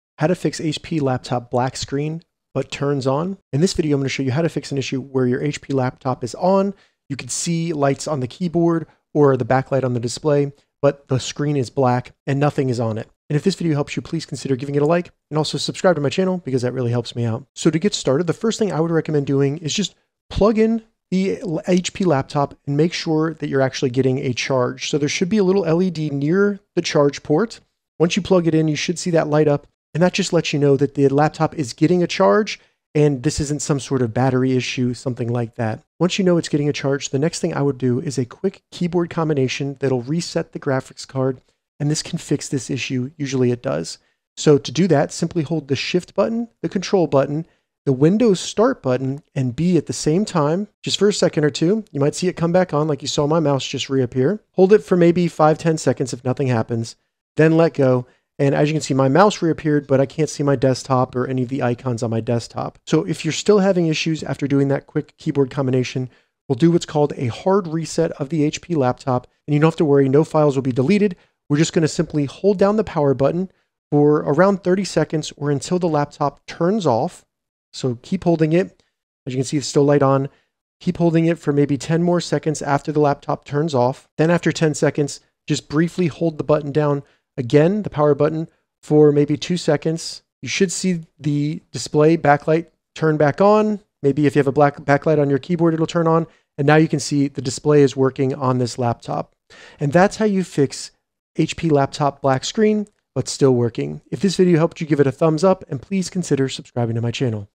How to Fix HP Laptop Black Screen But Turns On - 2025
How to fix HP laptop black screen but turns on? In this tutorial, I show you how to fix a HP laptop or pc that turns on with black screen. This means if your screen is black on a Dell laptop we can fix. To fix HP laptop black screen error we perform a hard reset of the computer. This tutorial should fix any HP laptop that turns on with black screen running Windows 10 or Windows 11.